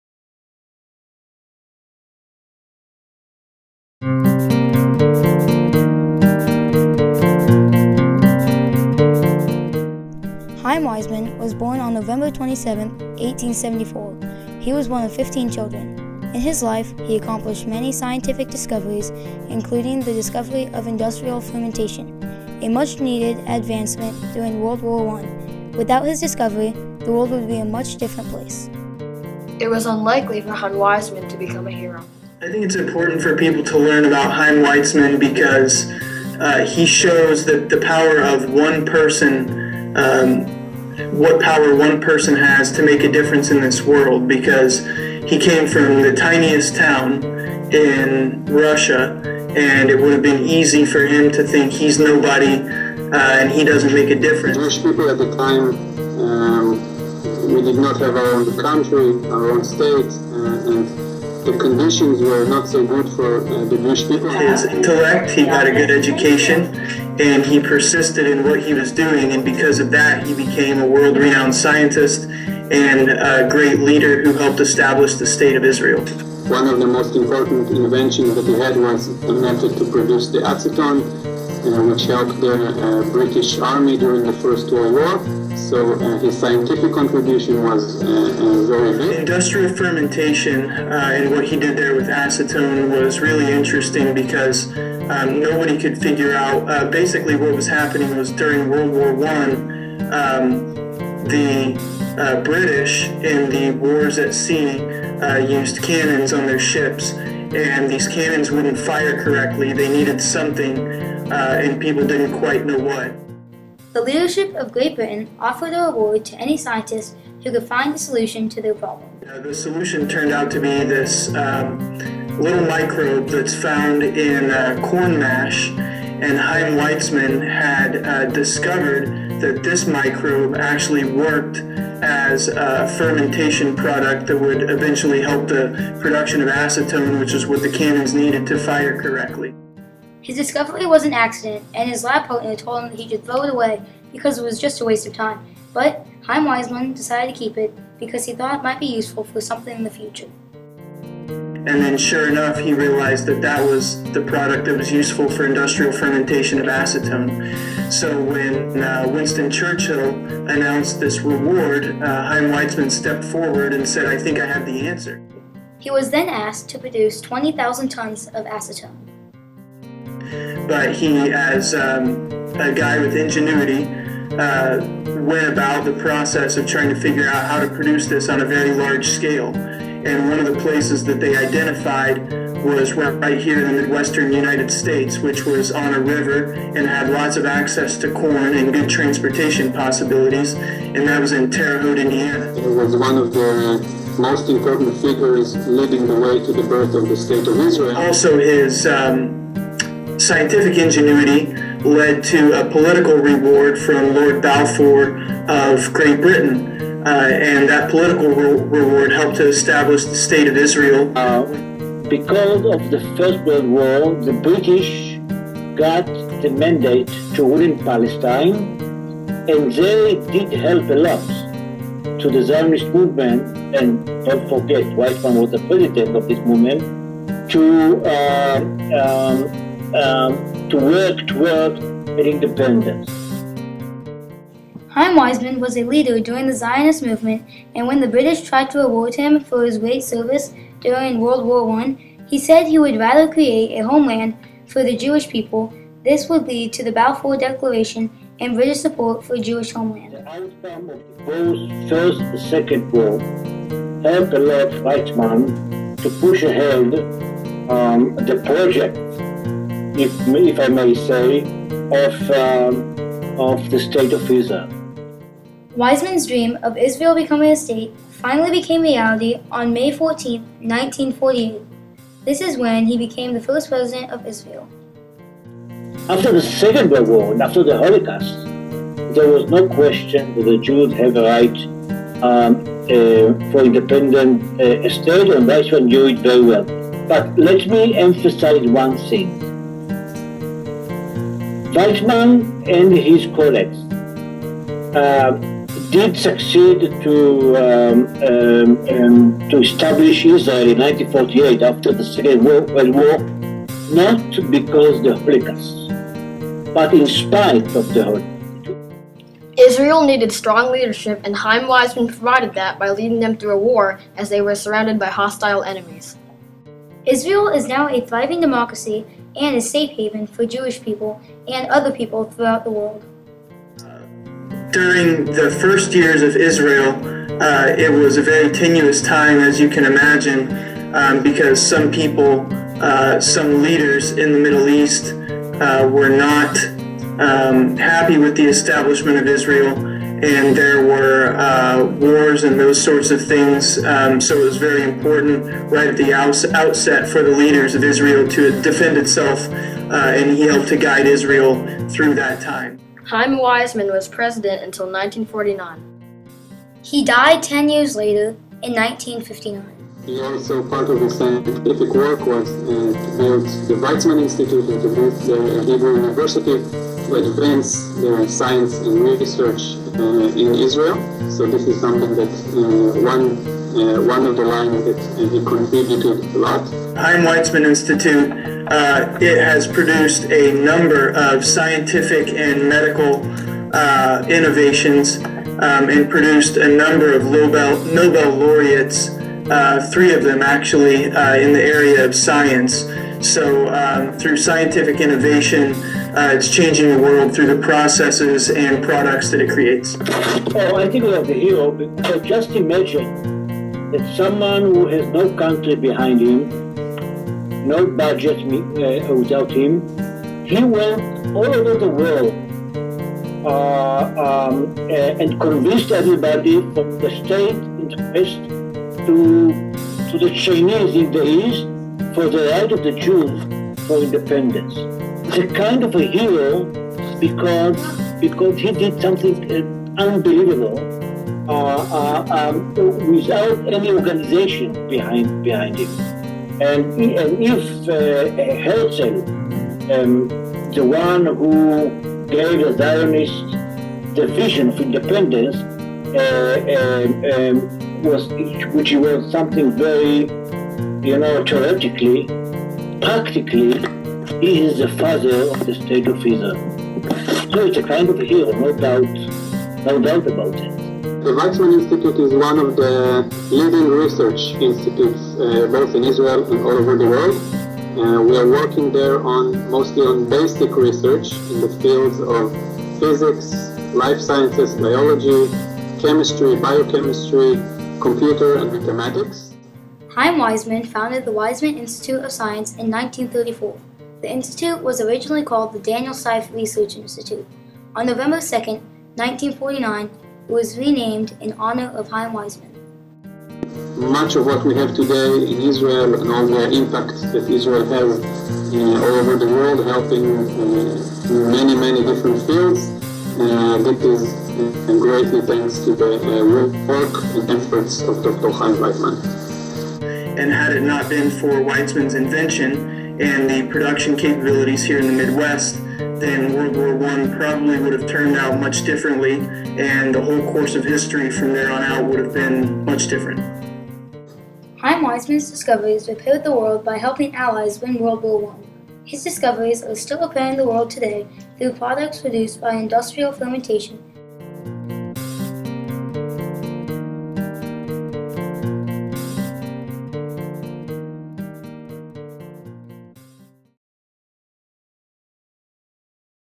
Chaim Weizmann: The Discovery that repaired the World
This is a documentary about Chaim Weizmann and his discovery that helped the Allies win World War I as well as his contributions to the formation of the nation of Israel. This is an entrant in the Lowell Milken Center's Discovery Award.